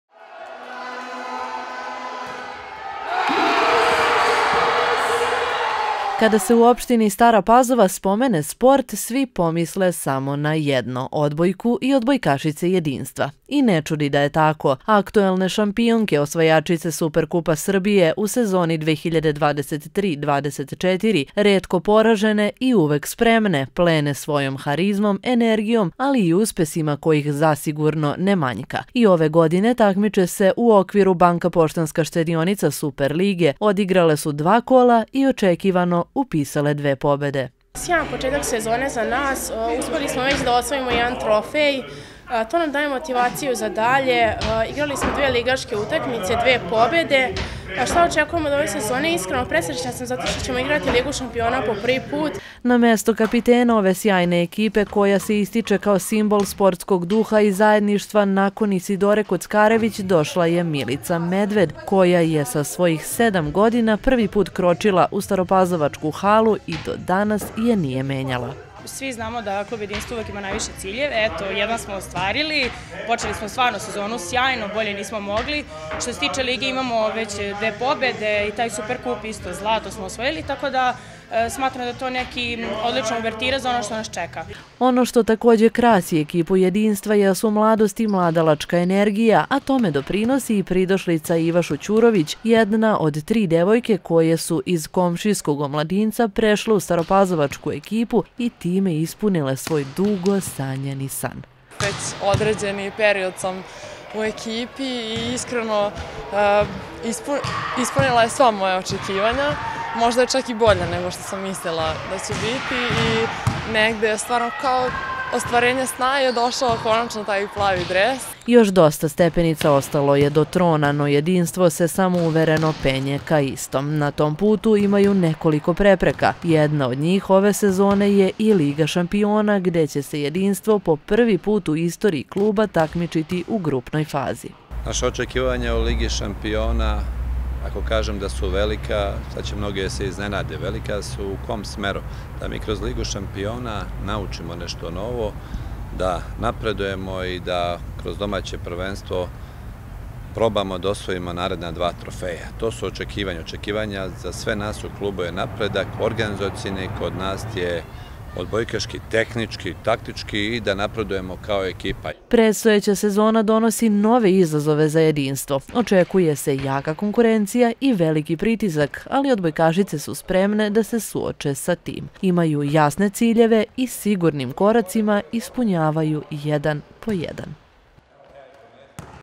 Odbojkašice Jedinstva Putevi do uspeha i očekivanja od predstojeće sezone
Odbojkaški klub Jedinstvo iz Stare Pazove jedan je od najuspešnijih sportskih kolektiva kako u opštini tako i u čitavoj zemlji. Nedavno je za njih počela nova sezona, a kakva su očekivanja od iste i kako su zadovoljni dosadašnjim učinkom u prilogu koji sledi..